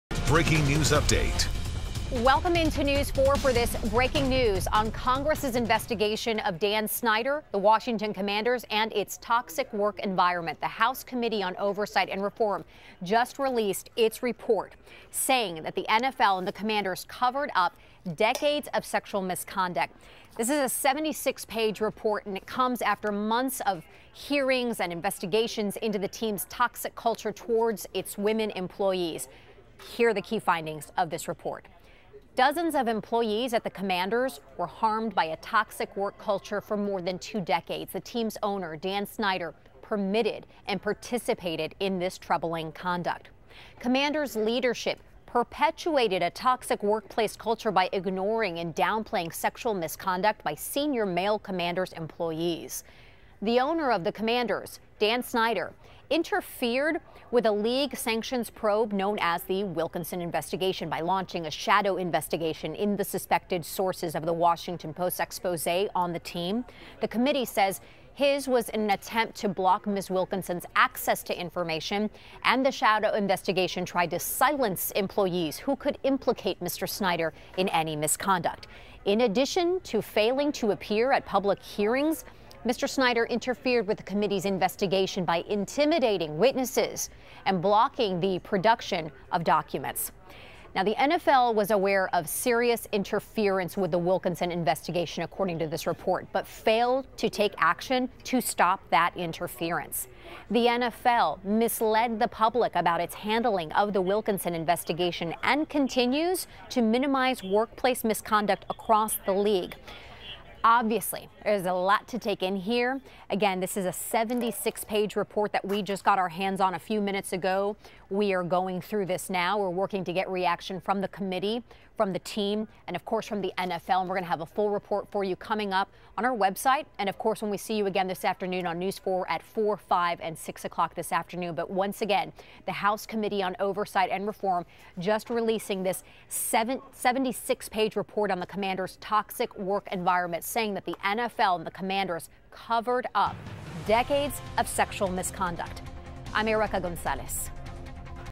Congress Releases Damning Report on 'Toxic' Washington Commanders Culture | NBC4 Washington
The Washington Commanders organization covered up decades of sexual misconduct and the team’s owner Dan Snyder tried to interfere with investigations into allegations of a toxic workplace, according to a scathing congressional report released Thursday.

The U.S. House Committee on Oversight and Reform released their 79-page report detailing findings from an investigation that lasted almost 14 months.